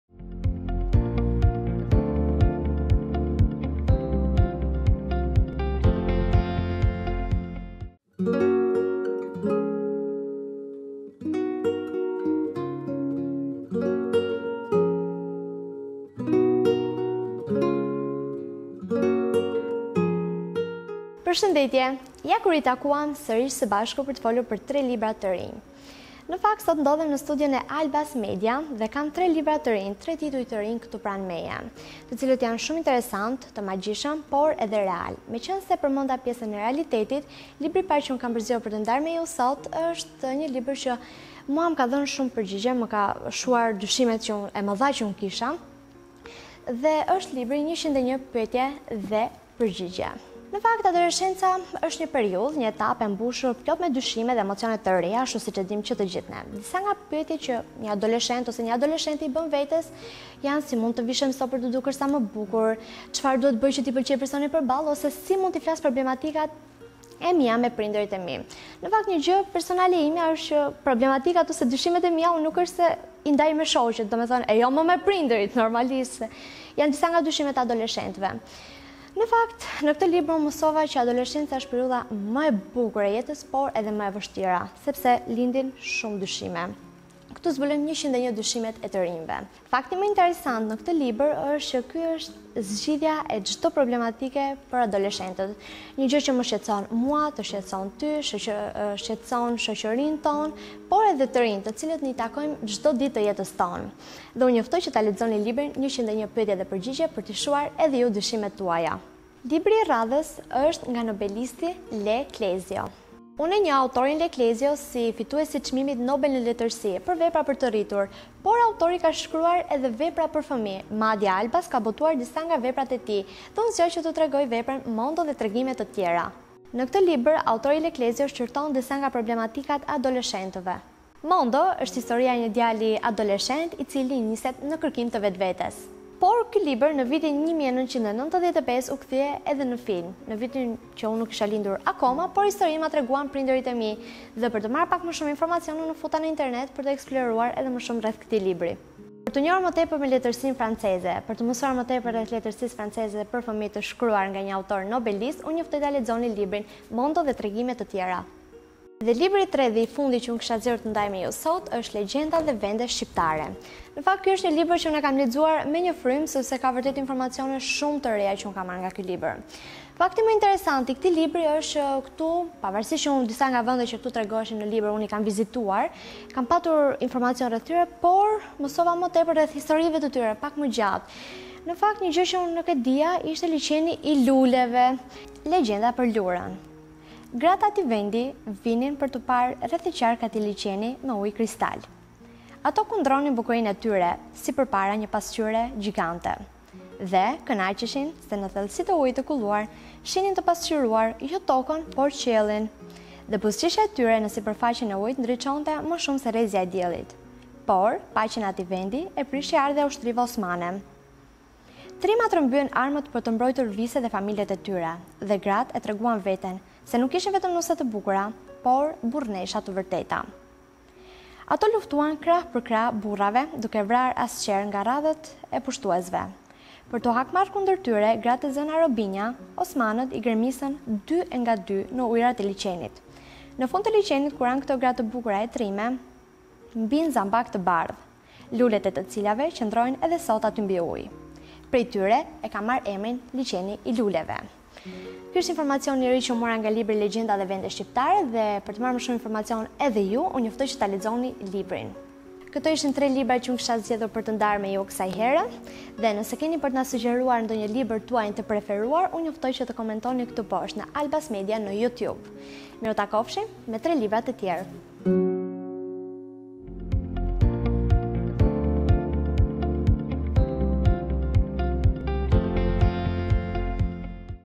Zbulojmë tre tituj të rinj me Klean
Këtë javë  Klea ka përzgjedhur tre libra interesantë. Ndiqni videon për të mësuar se për cilët tituj bëhet fjalë.